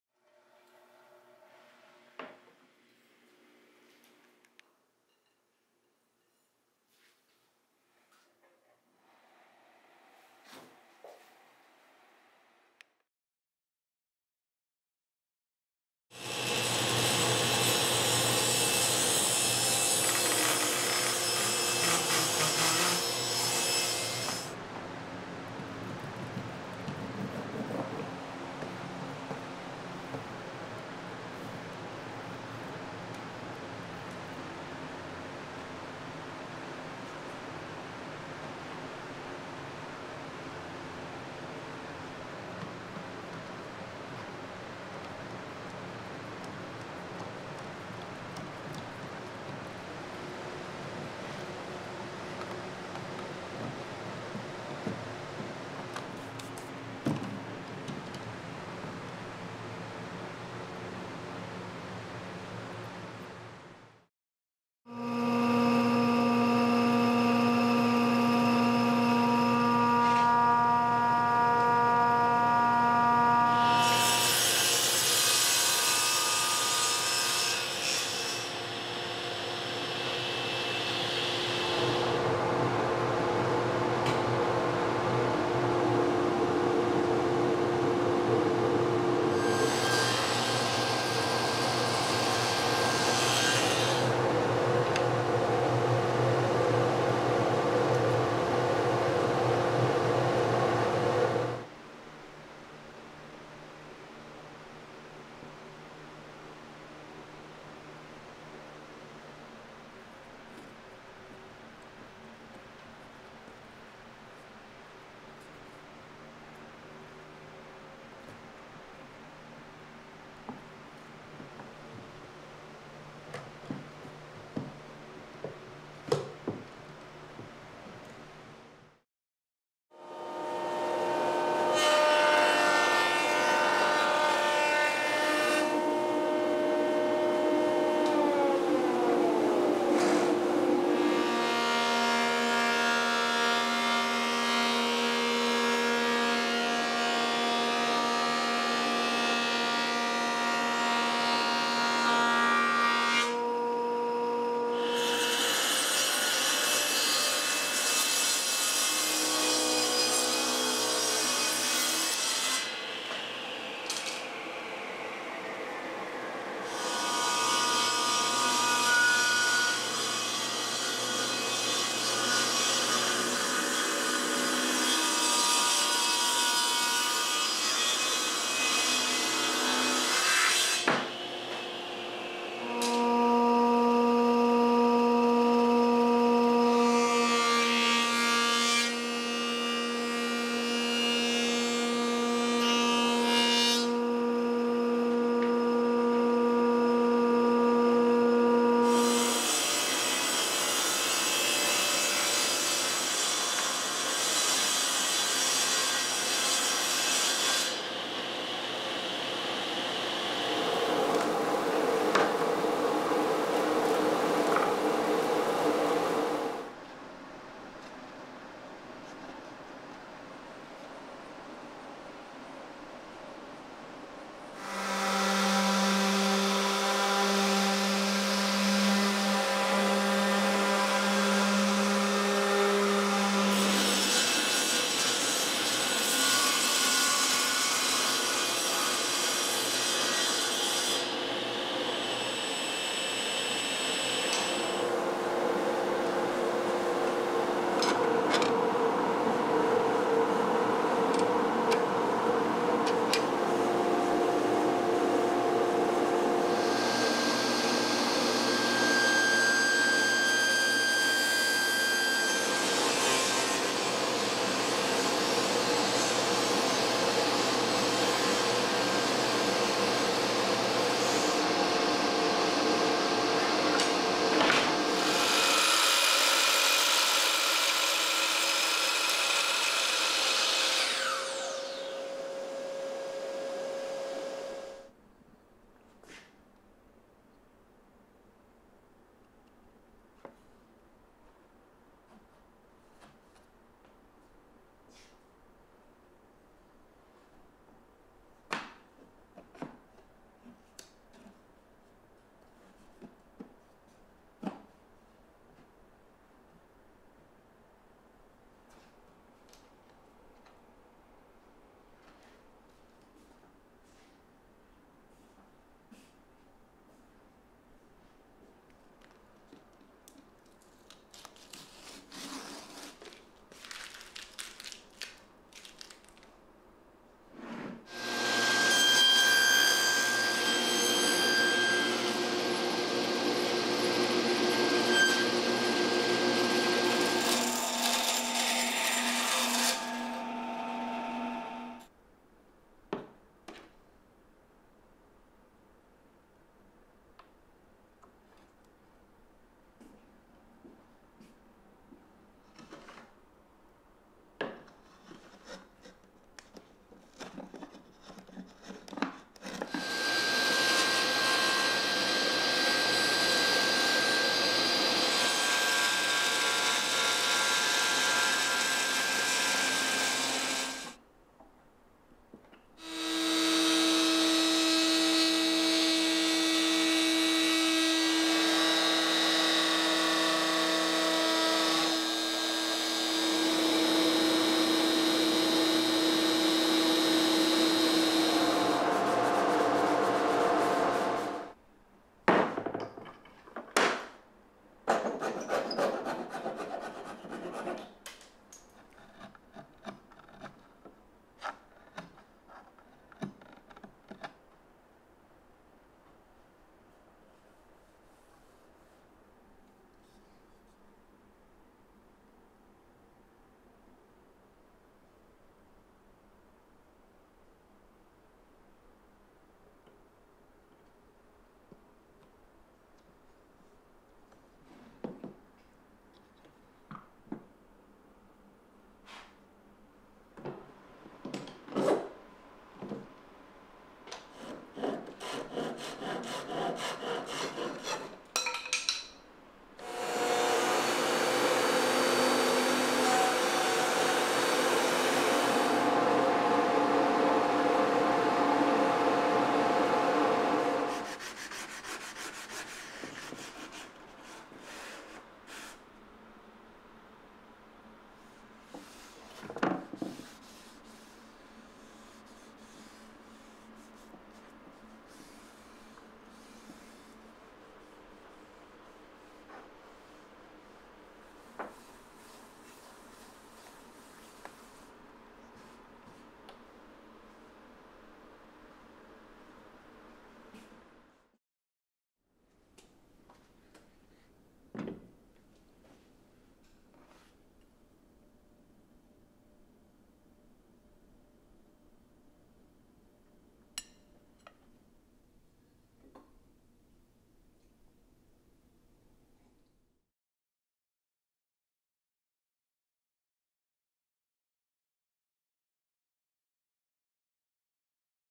[No Music] Wood Serving Trays | Woodworking Process 목공
Oh man, I've been wanting to make some wood serving trays for a while! ^_^

I actually quite like serving trays because I feel like they add a "completeness" to meals.  For example, you can have coffee and a croissant... but when you put it on top of a serving tray, it's like the experience transcends to another level.

And when I look around at serving trays, I notice a lot of handles are cut to be the shape the artist wants them to be - rarely, I see handles that are actually curved and bent to become the shape and that's what I wanted to achieve here.

I put the handles on the side as I feel it is more ergonomic and I used red oak to add a beautiful warmth to food.

The coffee featured is from Sawada in Chicago (my favorite), the adorable handmade ceramic cup is from Elenipal Ceramics (Athens, Greence), and the pastries featured were ABSOLUTELY delicious and from Mademoiselle Louise in Houston, TX!